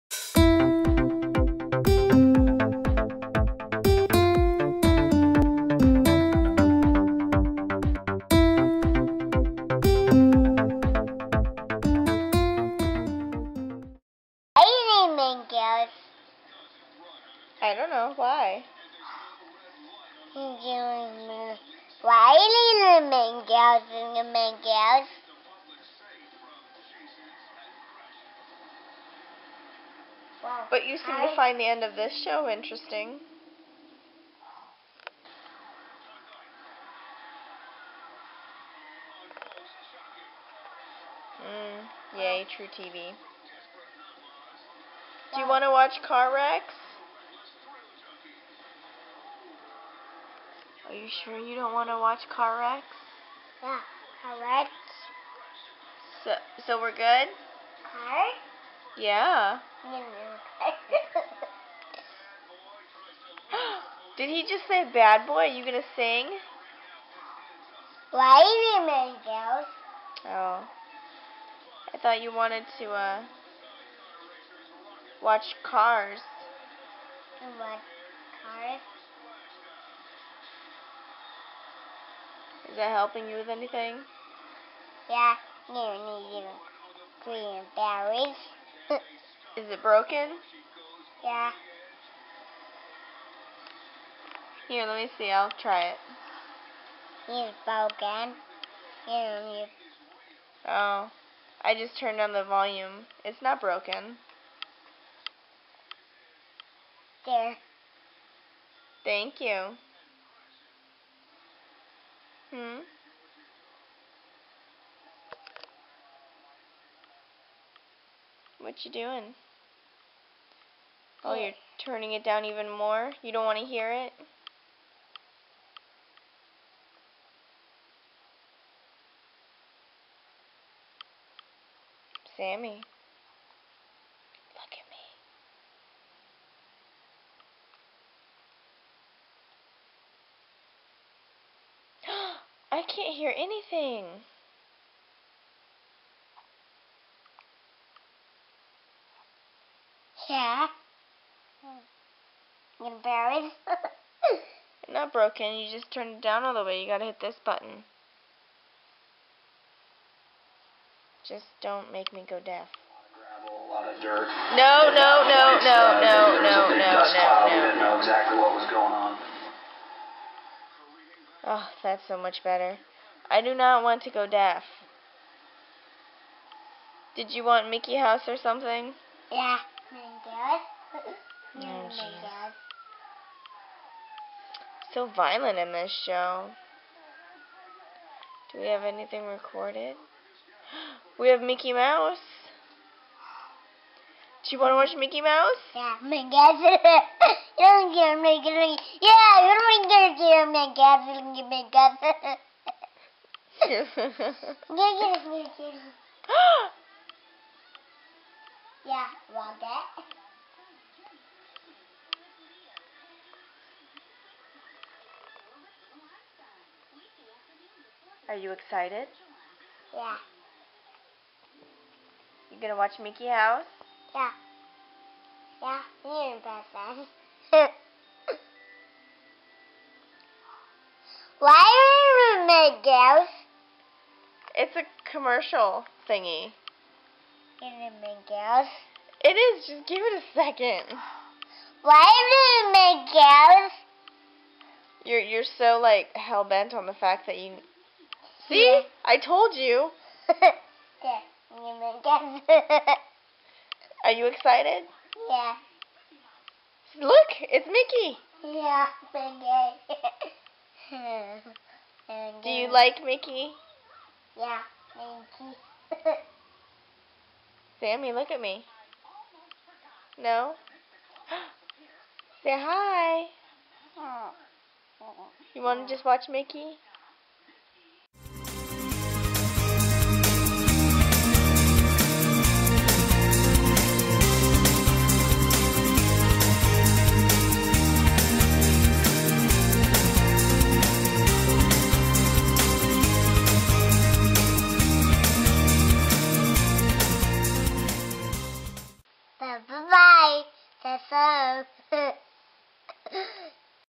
Search for Mickey House
Sammy wants to watch Mickey Mouse, but will he?

Filmed with Nikon Coolpix L26